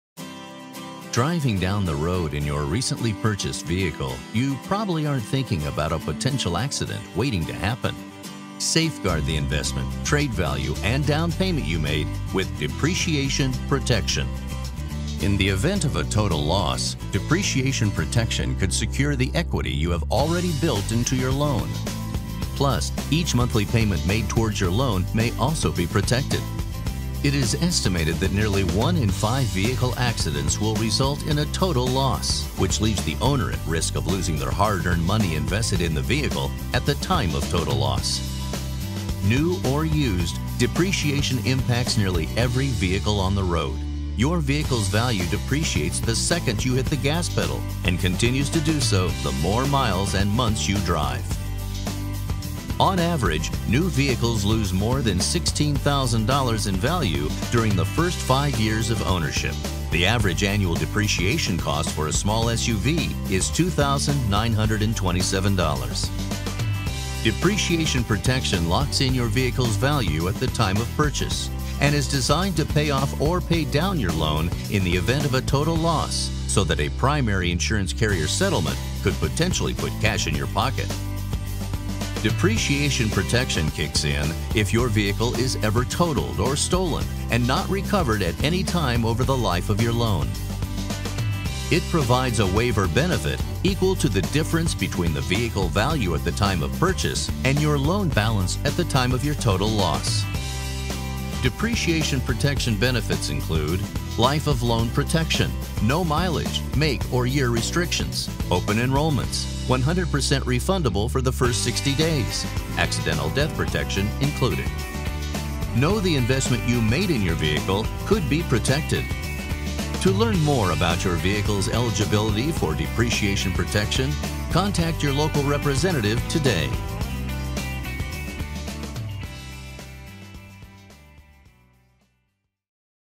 Depreciation Protection
Insurance companies wouldn't give back the money you put down on your vehicle if you were to get into an accident and total your vehicle.  DPW may be the solution to recoup a portion of the money that you have invested in your vehicle!